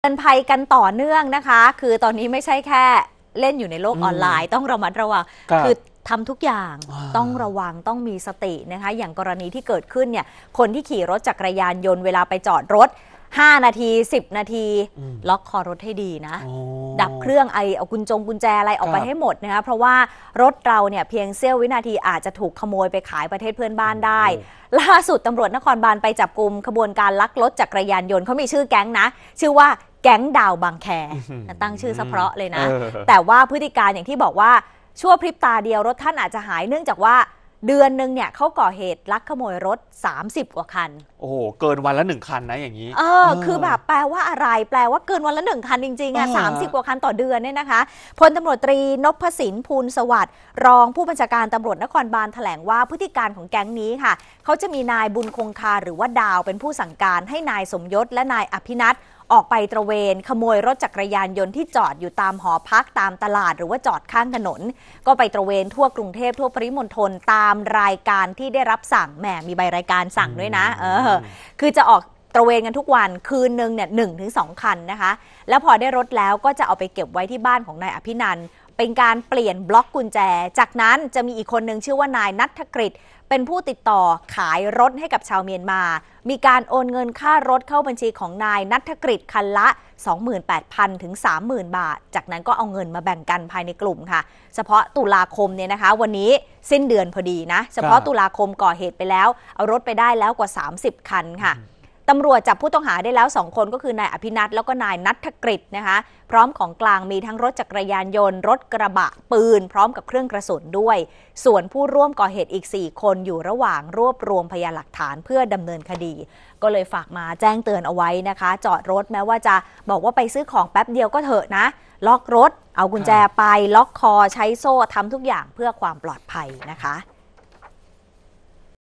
ตำรวจ จับ "แก๊งดาว บางแค" ตระเวนลักรถจักรยานยนต์ ข่าวค่ำ วันที่ 31 ตุลาคม 2566
ผู้ขับขี่รถจักรยานยนต์ก็ต้องระวัง อย่าจอดทิ้งไว้โดยไม่ล็อคคอรถ เพราะอาจถูกขโมยส่งขายไปยังประเทศเพื่อนบ้าน

ซึ่งล่าสุด ตำรวจนครบาล จับกุมขบวนการลักรถจักรยานยนต์ "แก๊งดาว บางแค" // แค่เดือนเดียวก่อเหตุทั่วกรุงลักรถไปกว่า 30 คัน